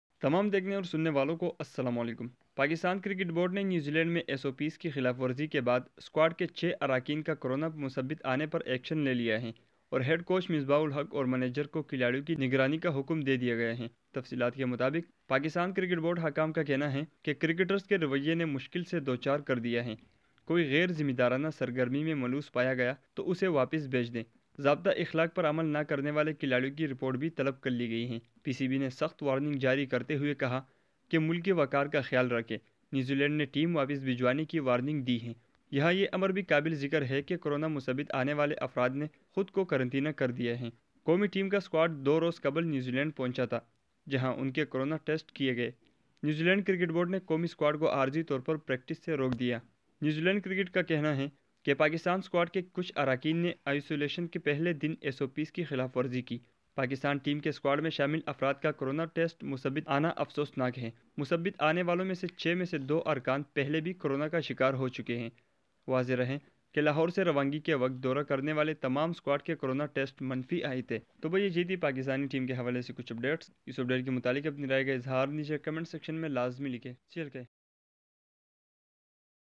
New Zealend CB Chairman Warns To Pakistani Team To Return
Matches
Schedule
Score Update
Cricket News
World Record